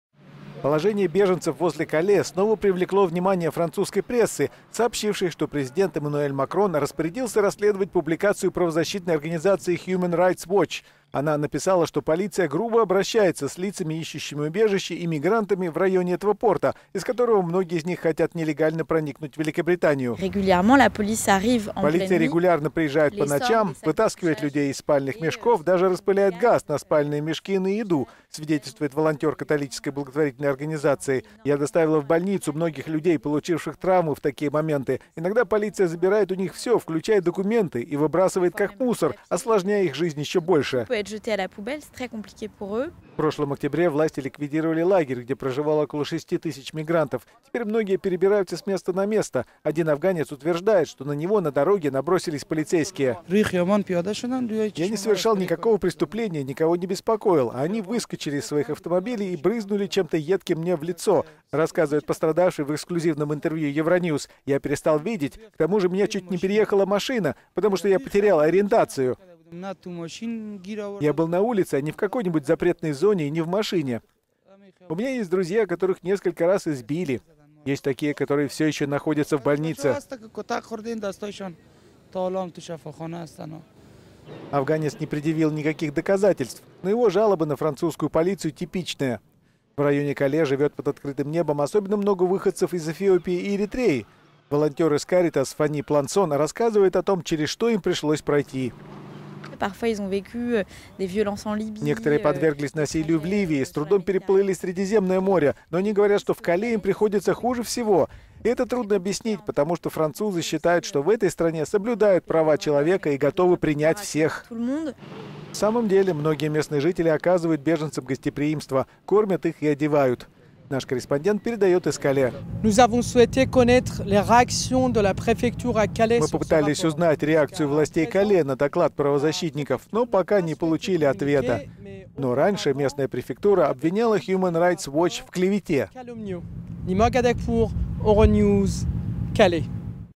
Мигранты в Кале жалуются на жестокость полиции
Положение беженцев возле Кале снова привлекло внимание французской прессы, сообщившей, что президент Эммануэль Макрон распорядился расследовать публикацию Human Rights Watch.
Эта правозащитная организация написала, что полиция грубо обращается с лицами, ищущими убежища, и мигрантами в районе этого порта, из которого многие из них пытаются нелегально проникнуть в Великобританию.

"Полиция постоянно приезжает по ночам, вытаскивает людей из спальных  мешков, даже распыляет газ на спальные мешки и…